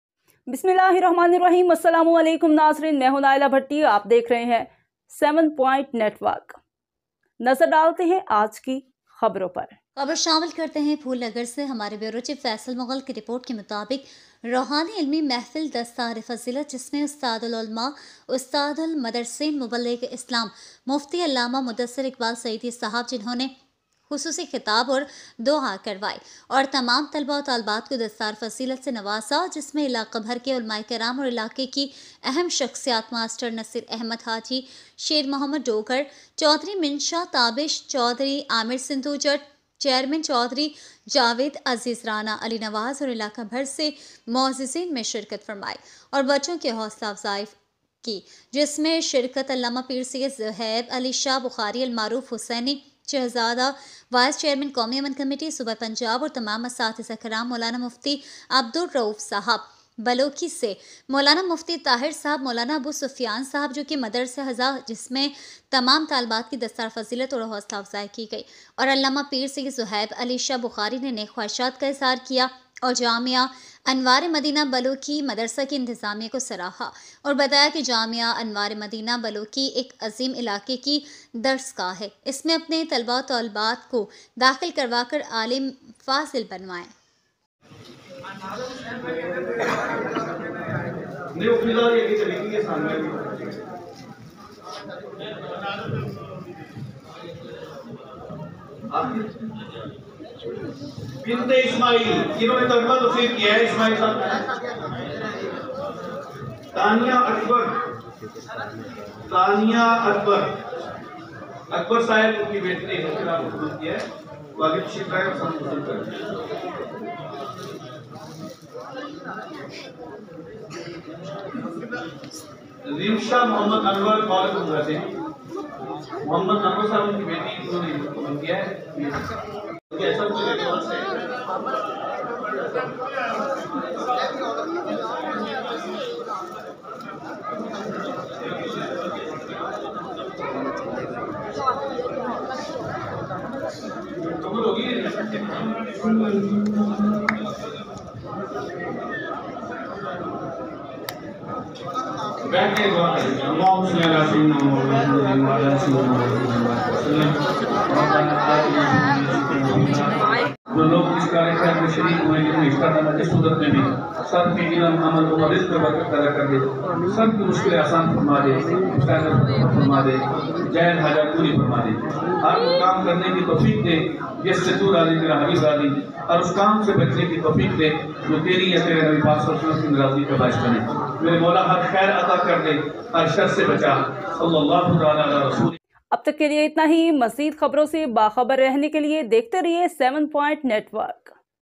Spiritual knowledge gathering Dastar Fazila is a school of great area of ​​Jamia Anwar Madina Balu.
The spiritual gathering of Dastar Fathizal,in which Ustad Ulama,Ustad Al Madrasin,the preacher of Islam,Mufti Allama Mudassar Iqbal Saeedee Sahib,who gave a special address and prayed and awarded Dastar Fathizal to all the students,including scholars from all over the region and important personalities of the region,Master Naseer.  Ahmed Haji Sher Muhammad Dogar Chaudhry Mansha Tabish Chaudhry Amir Sindhu JTT Chairman Chaudhry Javed Aziz Rana Ali Nawaz and dignitaries from across the region participated and encouraged the children in which Allama Pir Syed Zahib Ali Shah Bukhari known as Hussaini Prince Vice Chairman National Peace participated.Committee Punjab Province and all the representatives from Maulana Mufti Abdul Rauf Sahib Baloki to Maulana Mufti Tahir Sahib Maulana Abu Sufyan Sahib,which is a seminary in which all the students are encouraged and encouraged and Allama Pir Syed Zahib Ali Shah Bukhari expressed good wishes.He also praised the management of Jamia Anwar Madinah Balu Madrasah and said that Jamia Anwar Madinah Balu is a school of a great area and make all its students into scholars.7point network.